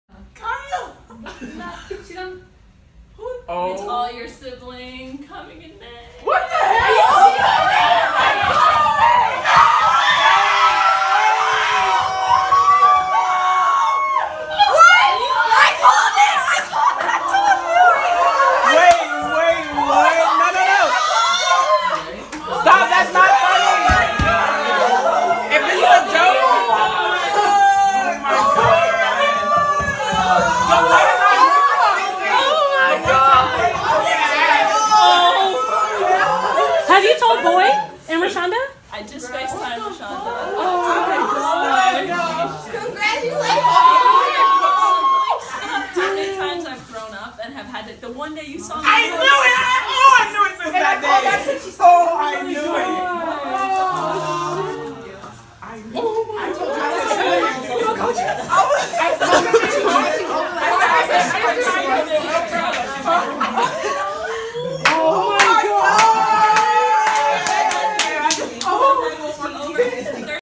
Cal's Lindsay Gottlieb Surprises Team With Pregnancy News
After practice Saturday, Cal coach Lindsay Gottlieb surprised her players by announcing she is pregnant.